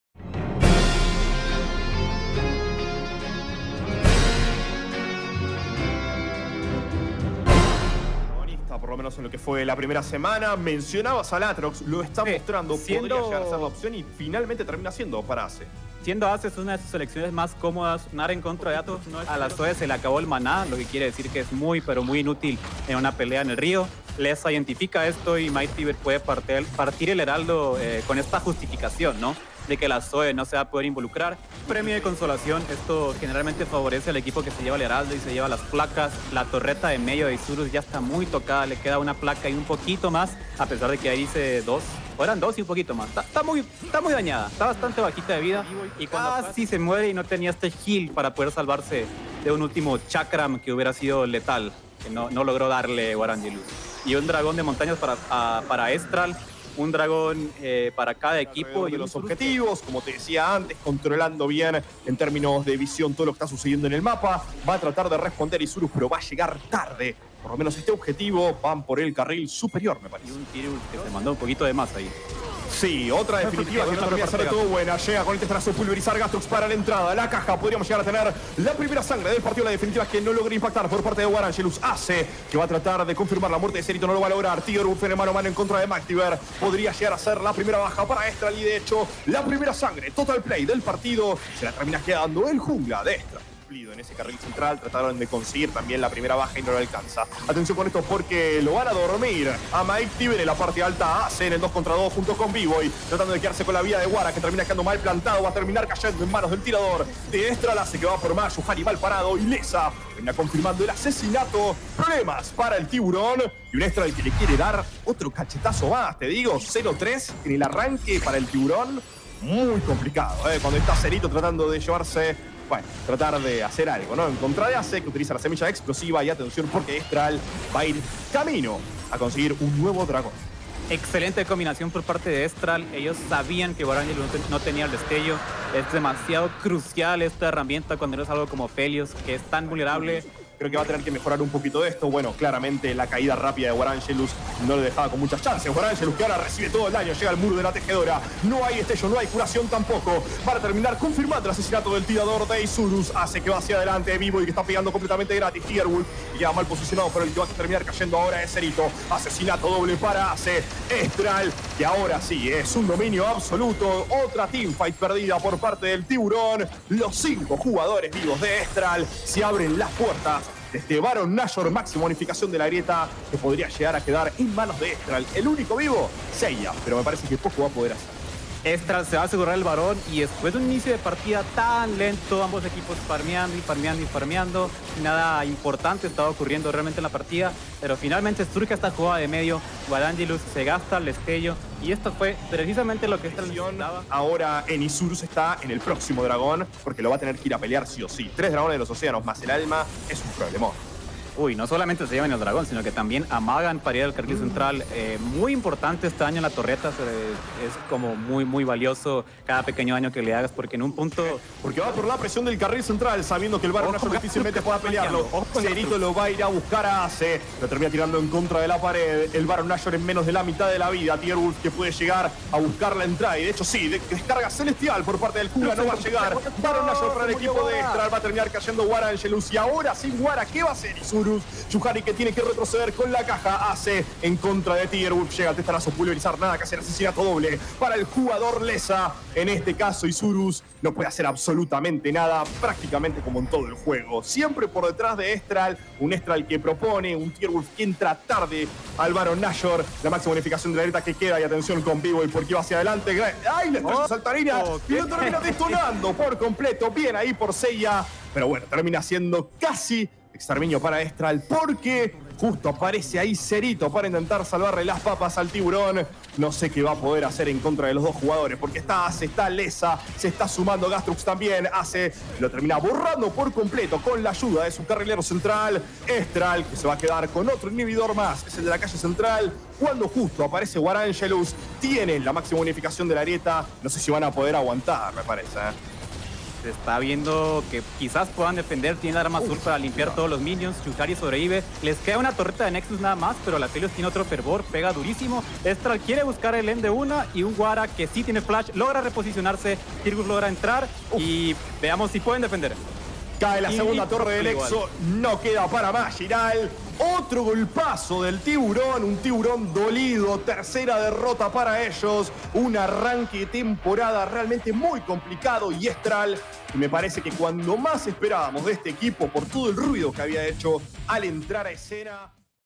LLA Apertura 2021 S2D1 | Isurus vs Estral Esports highlights | ISG vs EST
LLA Apertura 2021

¡¡VUELVE LA LLA!!

Isurus vs Estral Esports highlights LLA 2021.

Estral Esports:
Top ► Acce
Jungle ►  Mightybear
Mid ►  Leza
Botlane ► Bvoy
Support ►   Gastruks
Coach ►  ukFallen

Isurus :
Top ► Zero
Jungle ►  Tierwulf
Mid ►  Seiya
Botlane ► Warangelus
Support ►   Shu Hari
Coach ►  Yeti